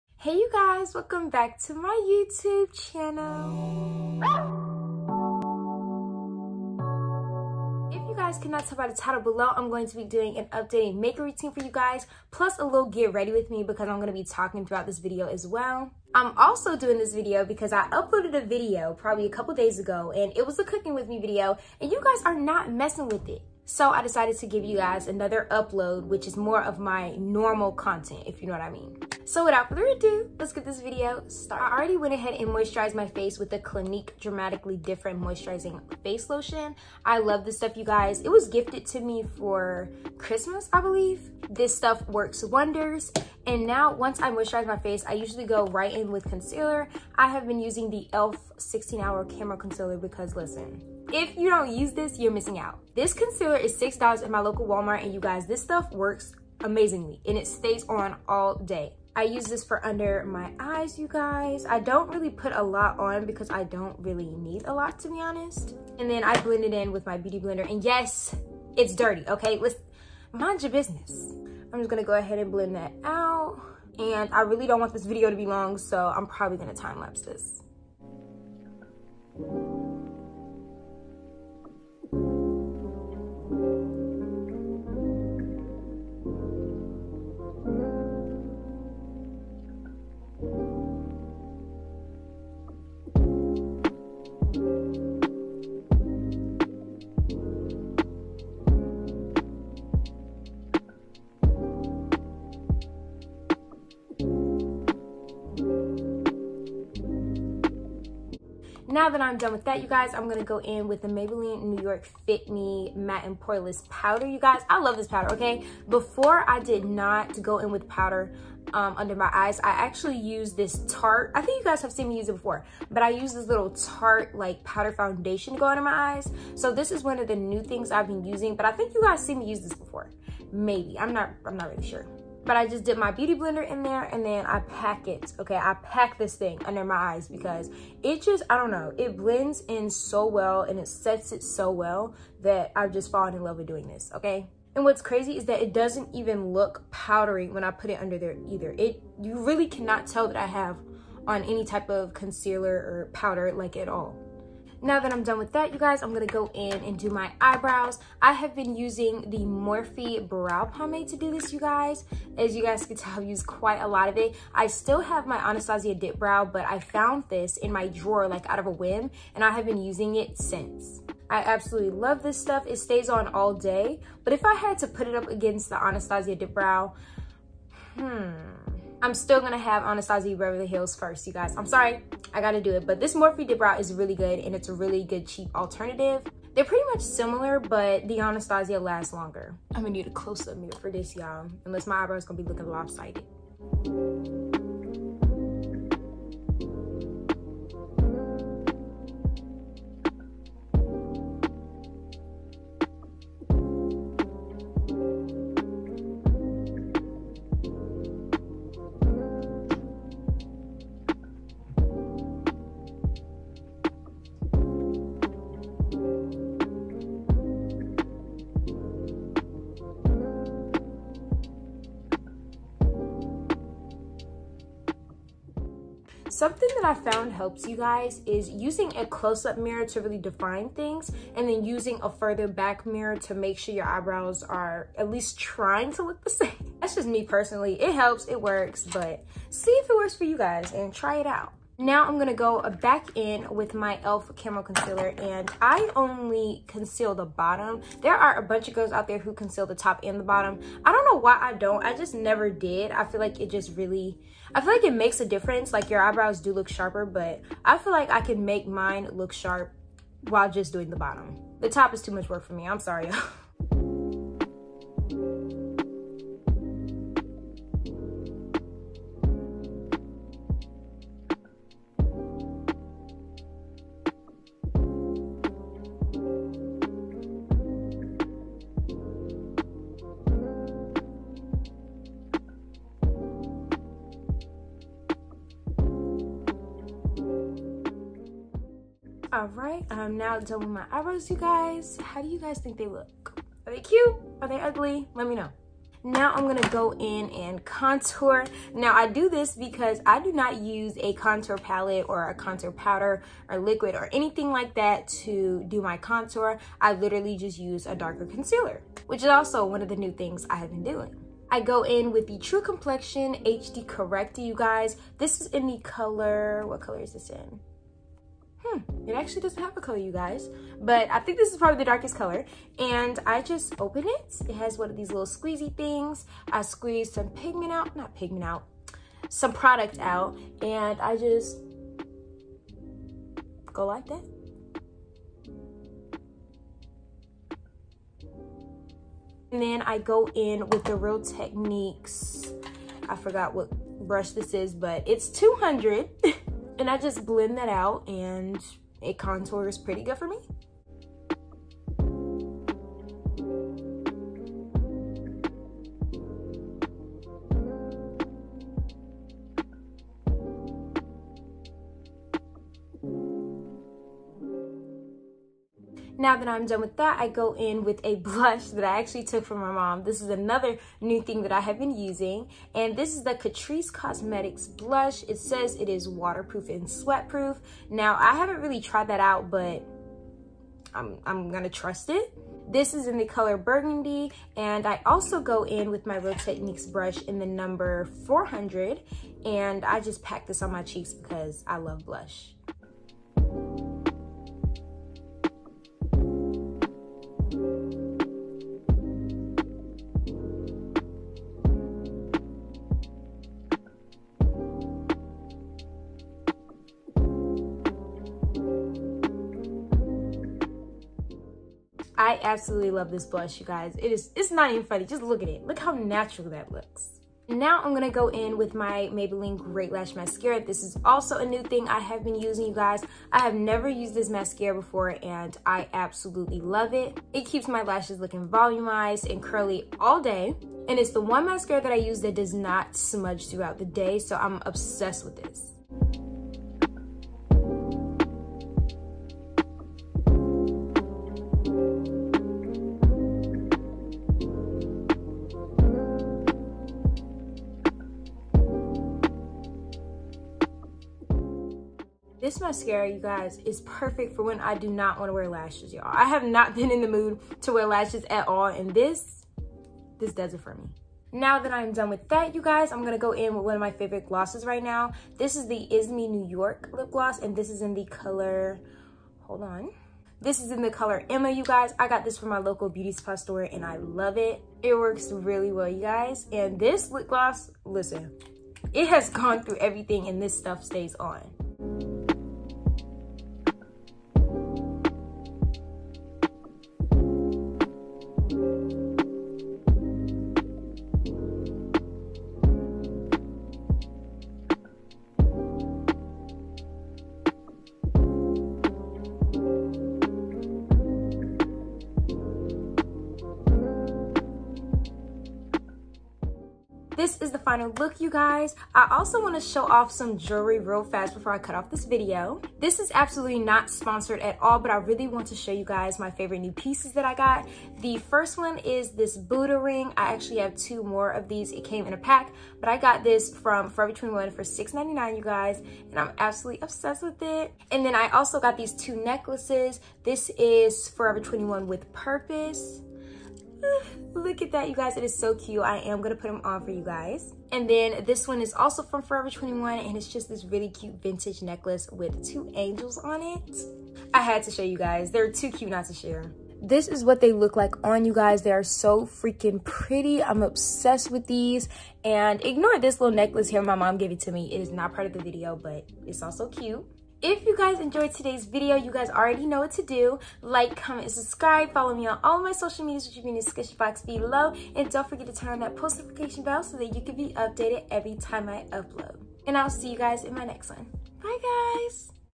my updated makeup routine 2022 | NO FOUNDATION MAKEUP ROUTINE
In todays video, I showed you guys my updated make a routine.💕💕 I use absolutely no foundation when it comes to my make-up routine!😁 IF YOU GUYS ENJOYED TODAYS VIDEO!!